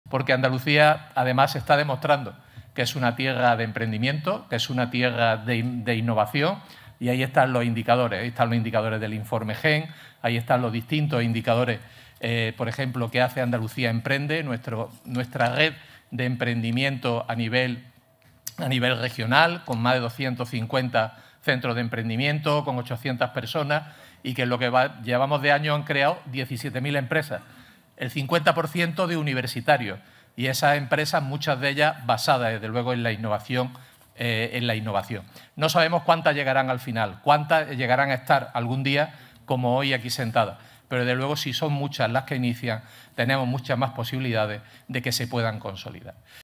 Villamandos destaca el emprendimiento innovador como “signo de identidad” de Andalucía
El consejero de Universidad, Investigación e Innovación de la Junta de Andalucía, José Carlos Gómez Villamandos, ha destacado este lunes que el emprendimiento innovador es ya "signo de identidad" de Andalucía, donde desde hace algunos años surgen en todas las provincias empresas de valor y con base en la innovación, "piezas fundamentales para consolidar un tejido empresarial competitivo y con futuro".